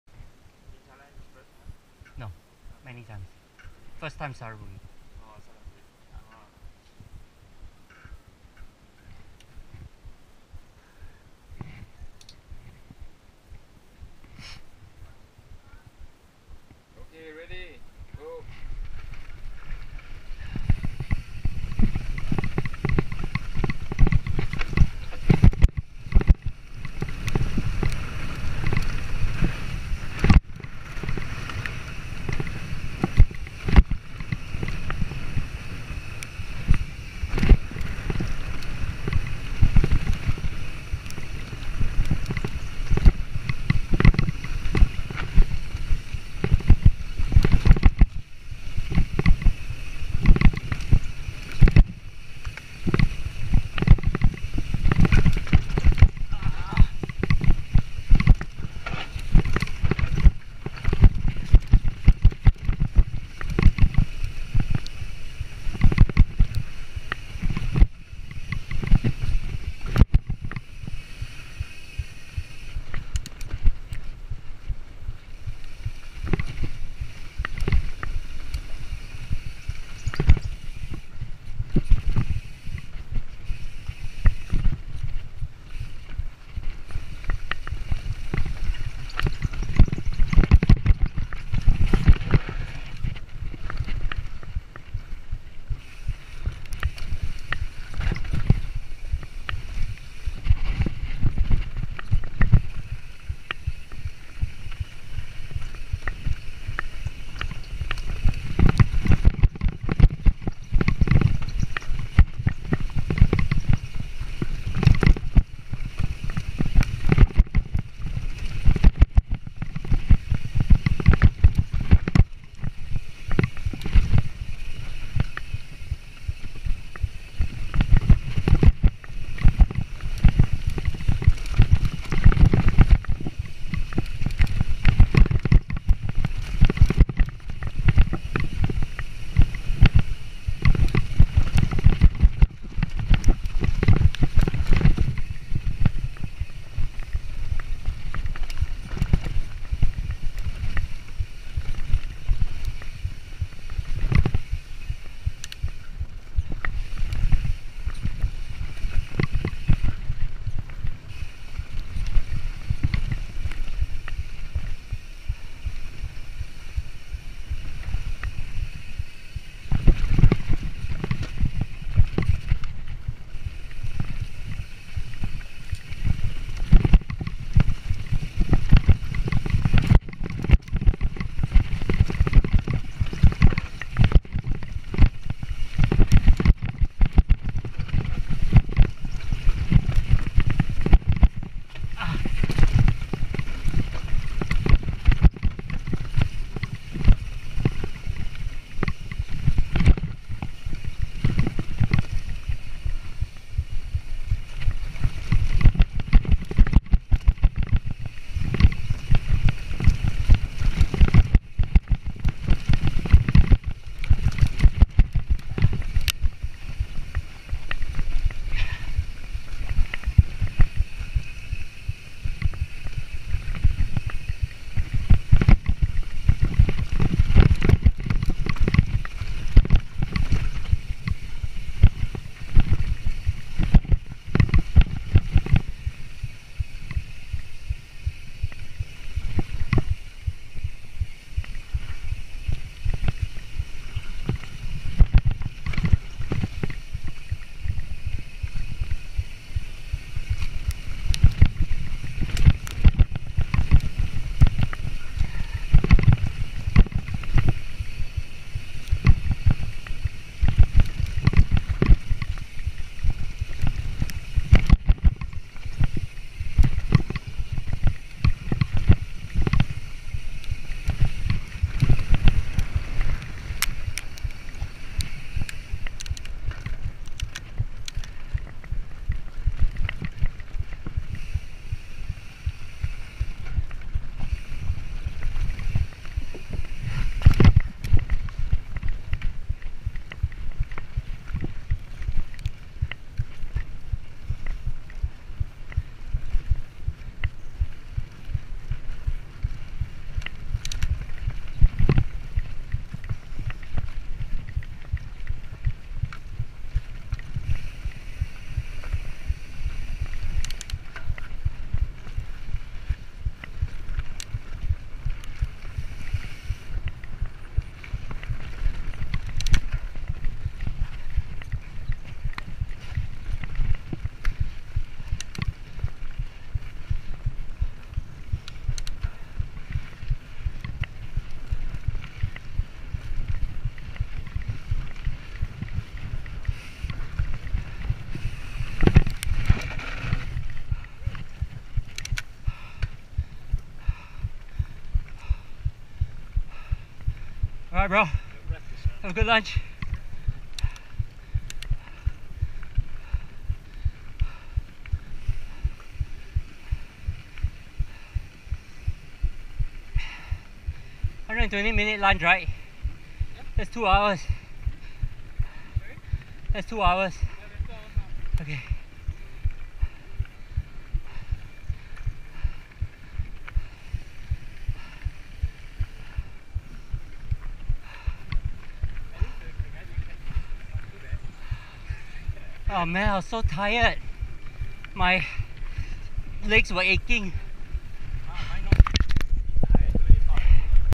Thailand Enduro Series - Rd 2 - Saraburi - Special Stage 2
SS2 of Thailand Enduro Series adventure in Saraburi. Lots of roots, natural features, small rock gardens and twists and turns. One of the longer enduro tracks at this event, this track proved to be my nemesis. I do wish i spent a little more time riding it.